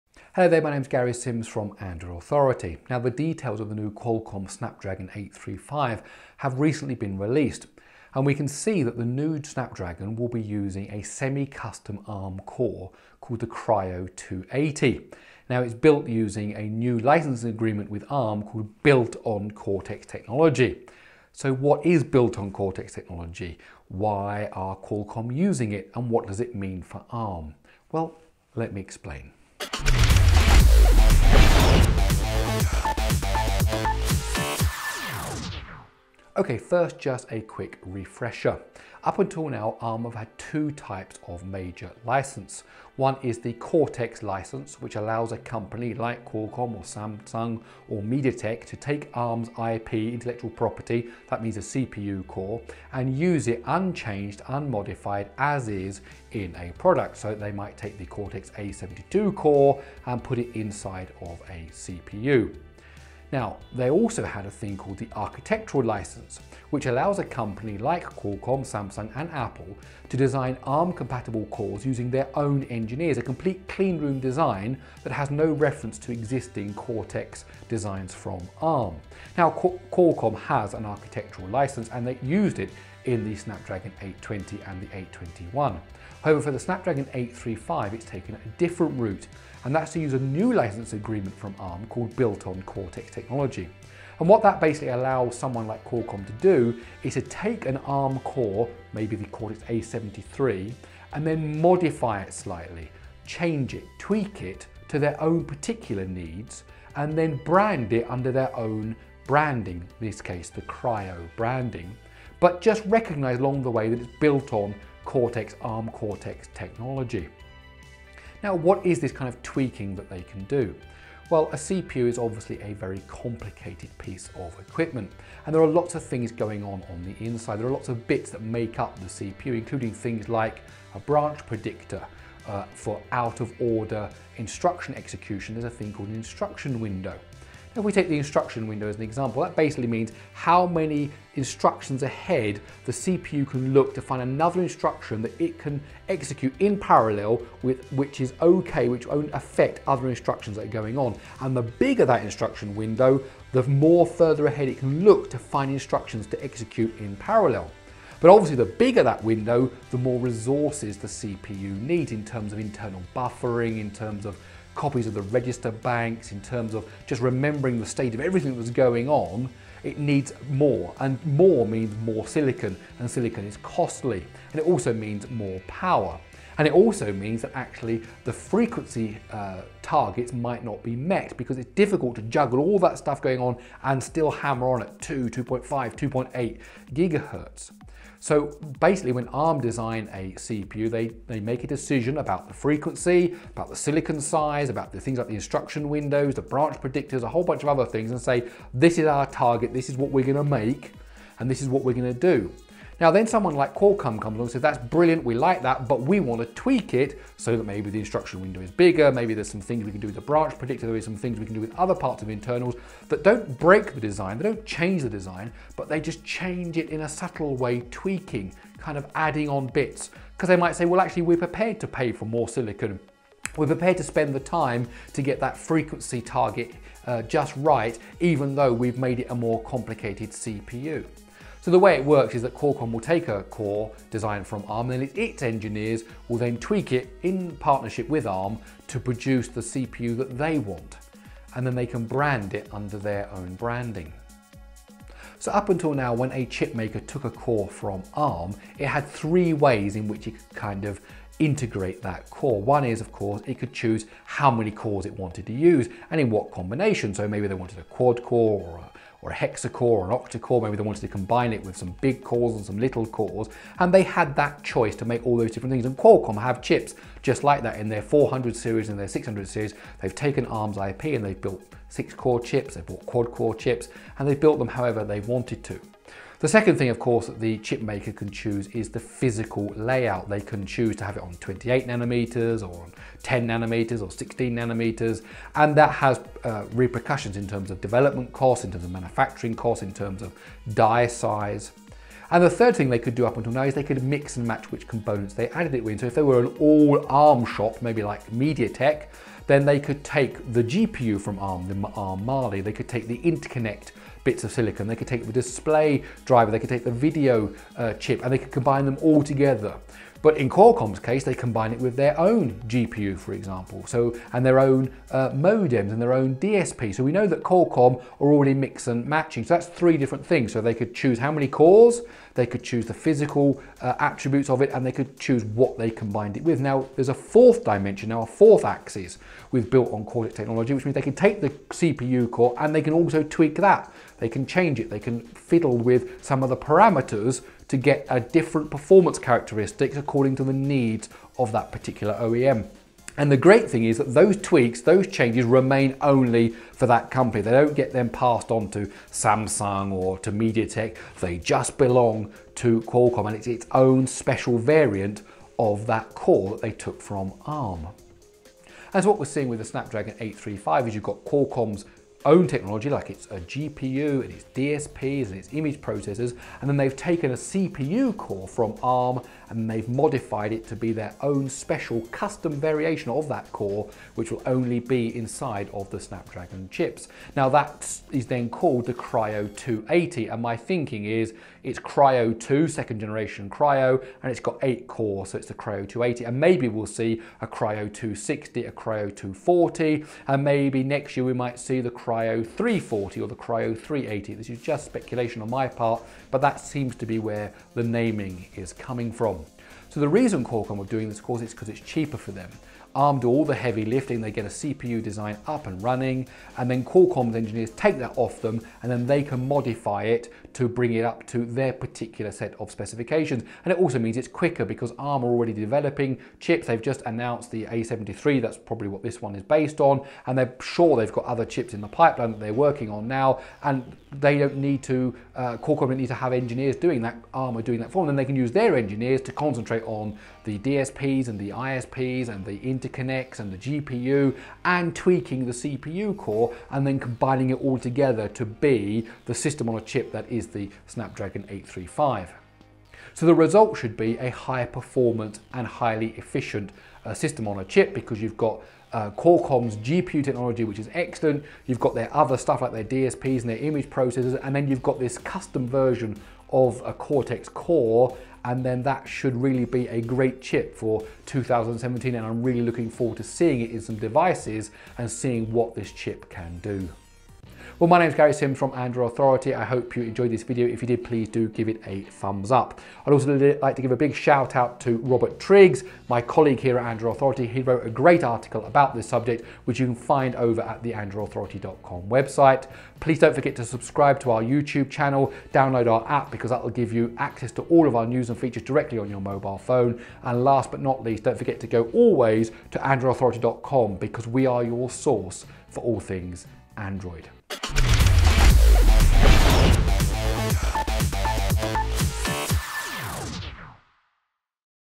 Snapdragon 835 and "Built on ARM Cortex Technology" - Gary Explains
The new Kryo 280 CPU inside Qualcomm's new Snapdragon 835 is the first mainstream semi-custom CPU core design "built using ARM Cortex technology." Gary explains what it all means.